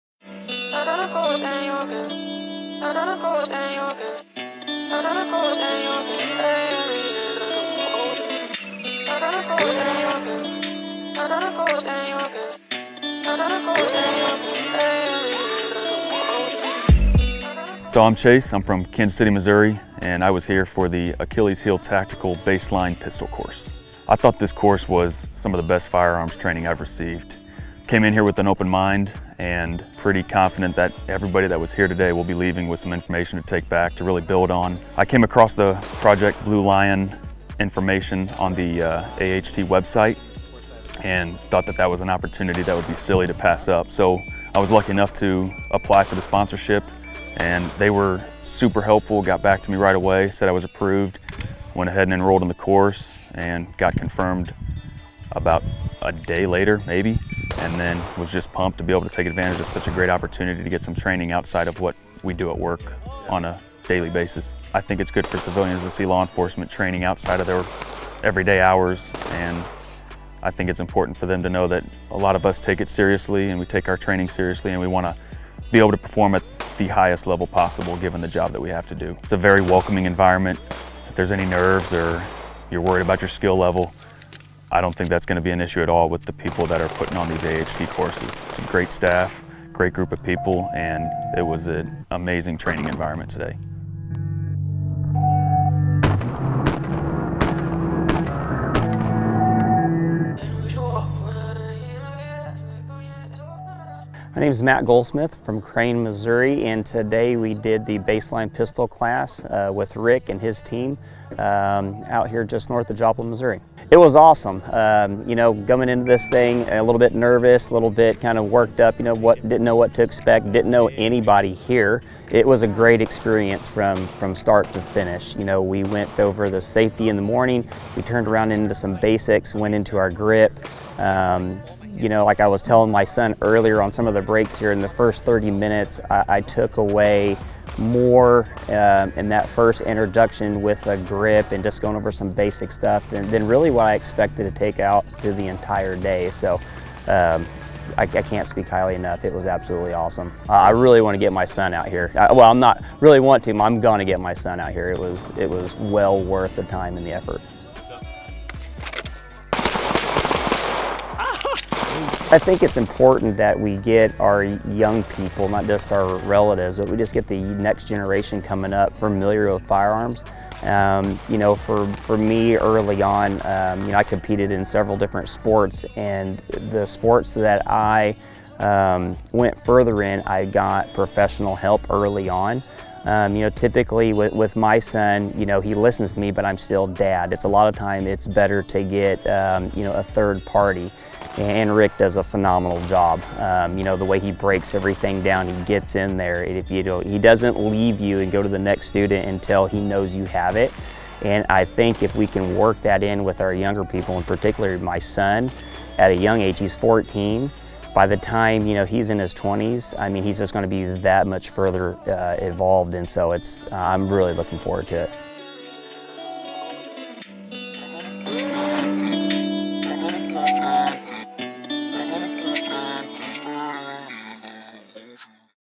BASELINE PISTOL IN JOPLIN, MISSOURI
As I travel across the nation I am fortunate to meet some amazing individuals that invest their time & assets into my training company. I want their voices to be heard, to share with all of you what they thought of the training they received. Here is some insight on what is being said about the training curriculum at Achilles Heel Tactical. Meet Chase a Project Blue Lion LEO recipient & Matt Goldsmith who share their reasons for attending Achilles Heel Tactical training courses.
Thank you for this review Chase & Matt and thank you for being a part of this new series.